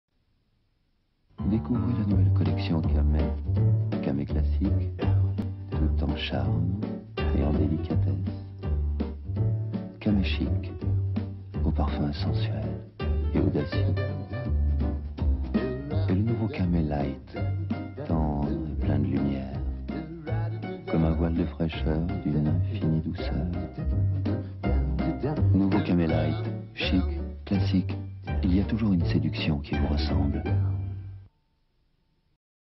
uomoclassico.com - Jesse Harris & Christy Turlington, Camay Commercial directed by Arthur Elgort
- Jesse Harris & Christy Turlington, Camay Commercial, directed by Arthur Elgort.